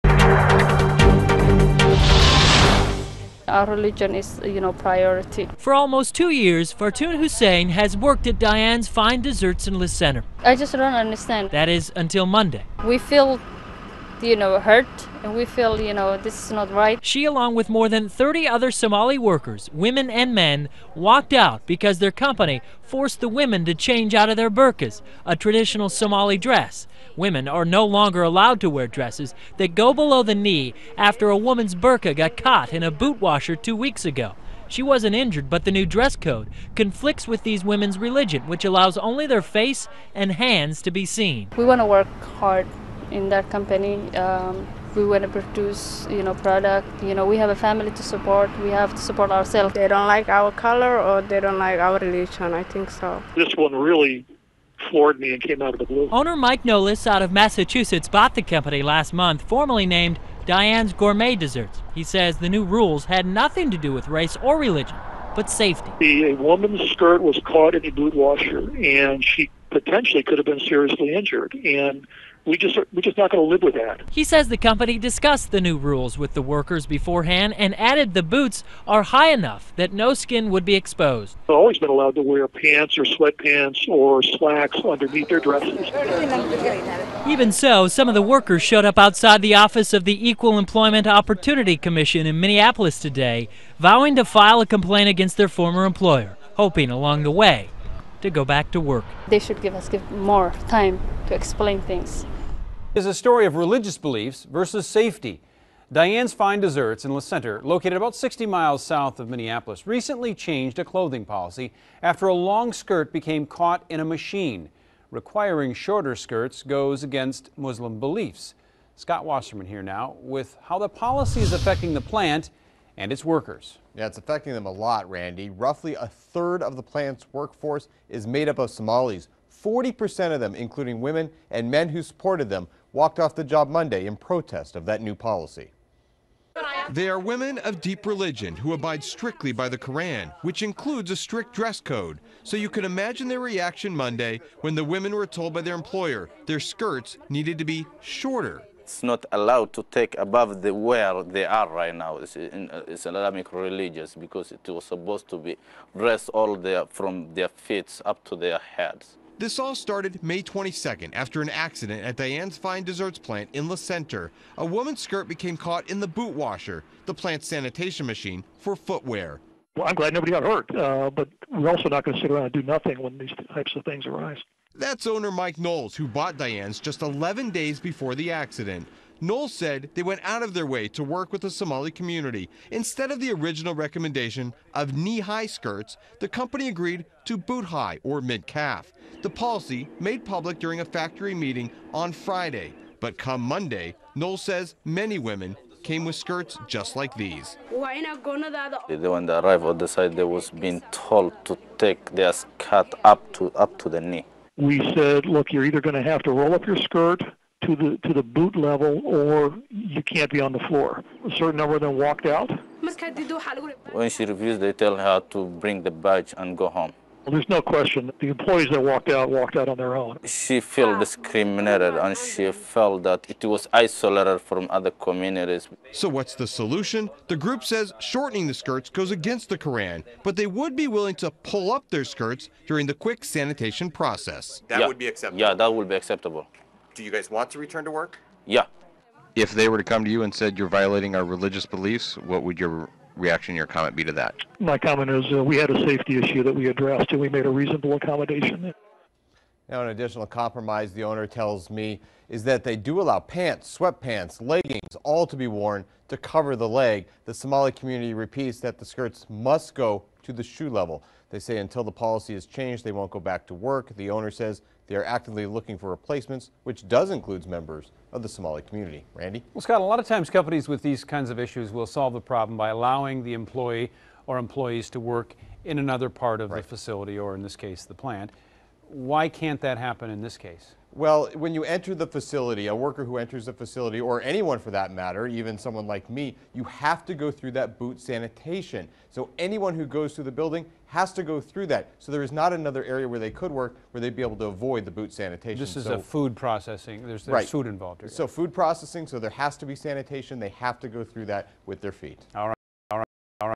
Somali workers walk out of Minnesota bakery in protest of new dress code.
More than 30 Somali employees at a bakery walked out in protest Monday after the owner ordered female workers to wear mid-calf skirts after a woman's long dress recently got caught in a boot washer..

Mike Knowles, the owner of Dianne's Fine Desserts, told the Faribault Daily News that the new guidelines were installed for safety concerns. Somali workers, however, call the new rules an attack on their Muslim religion.

The women reportedly showed up to work in their full-length attire and were given the option to adjust their skirts or leave. About a dozen women left and they were joined by 20 men who say the dress code goes against teachings in the Koran.